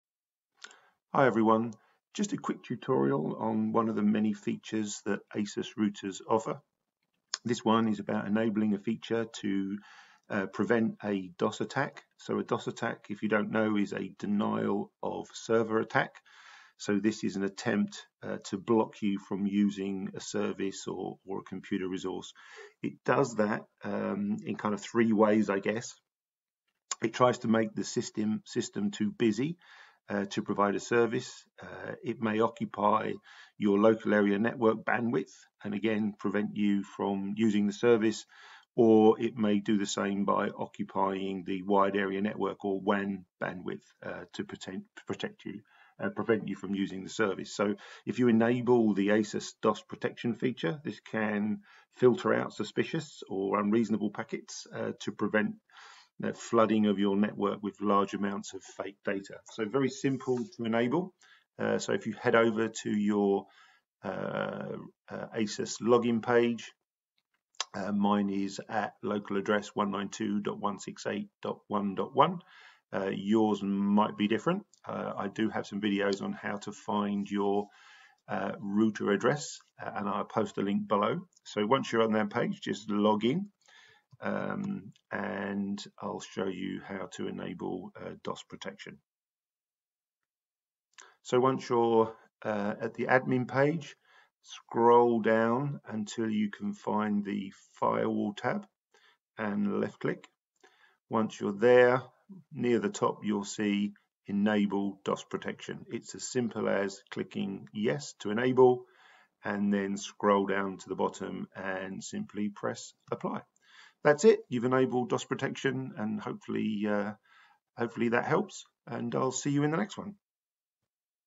How to Enable ASUS DoS Protection
A short video of how to easily setup the DoS attack protection feature on an ASUS router and prevent your network from being flooded by large amounts of fake traffic.